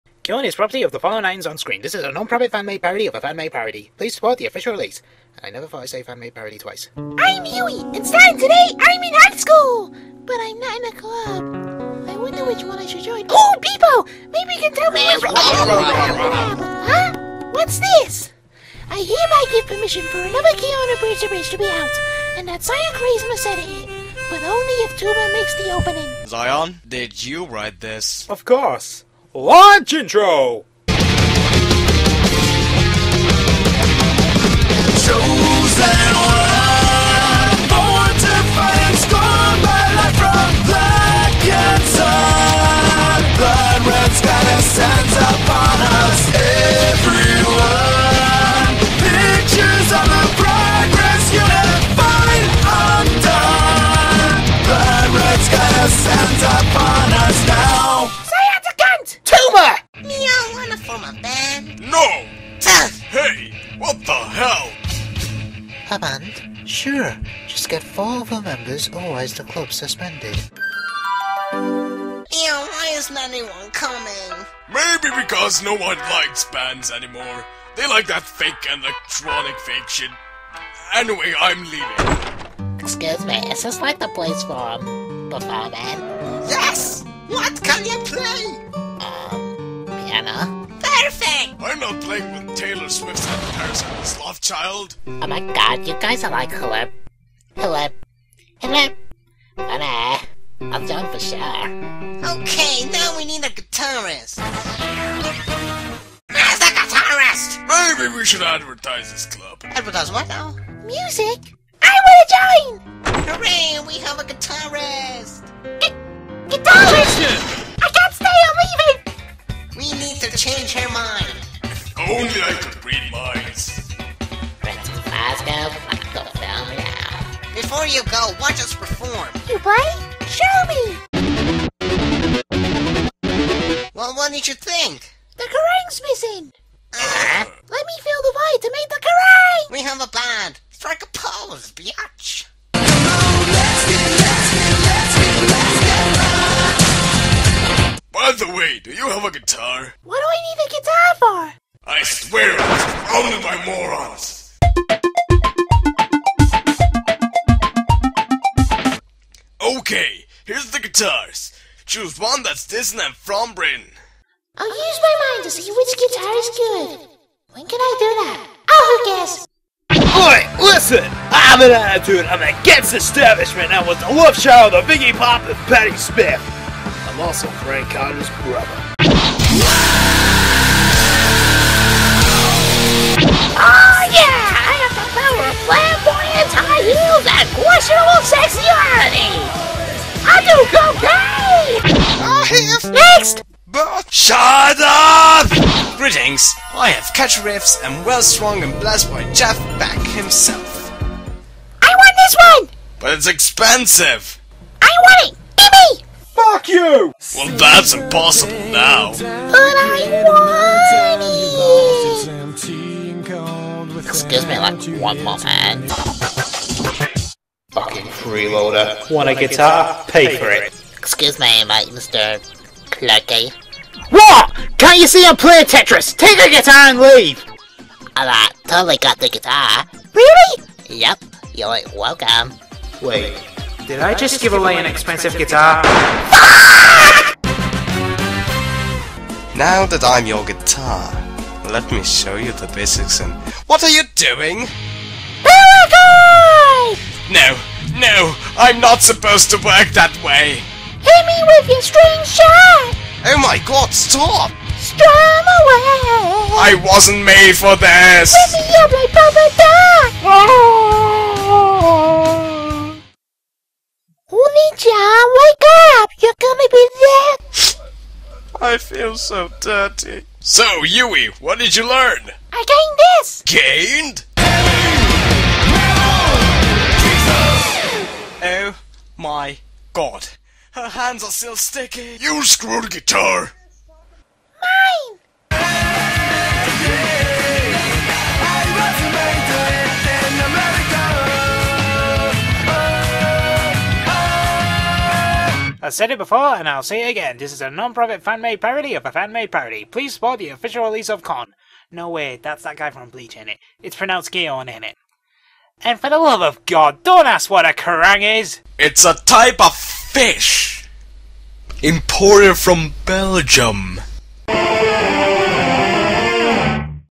KAA Just5Guys Tribute
PLEASE READ

Who says Just4guysandachick can only do this? From the KRAZEY minds of ZionKraze and TumorGX, comes an Abridged Abridged of [censored] originally by Just5Guys, destroyed by two SMFs.

Written by ZionKraze & TumorGX
Opening by TumorGX
Edited by ZionKraze

This is nothing but a Fan Made Parody of a Fan Made Parody.